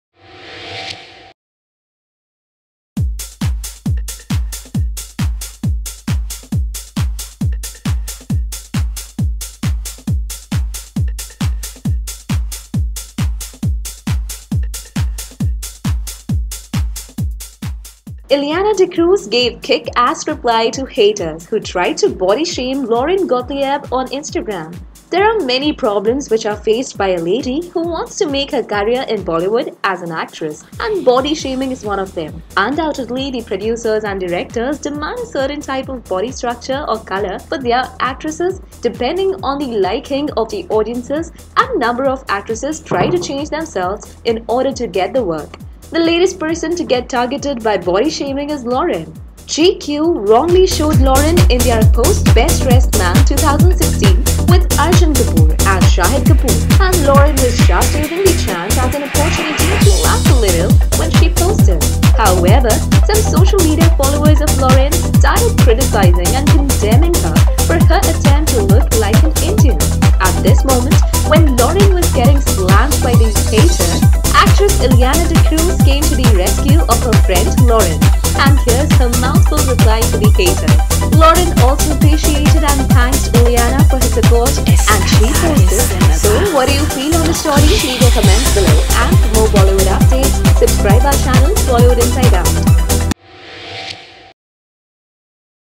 Ileana D’Cruz slams haters for body shaming Lauren Gottlieb| Bollywood Inside Out
Lauren was criticized by a few haters saying she 'looked like a man' and another who ridiculed her for 'trying to look Indian Ileana D'Cruz came to her rescue,Check out the story for more details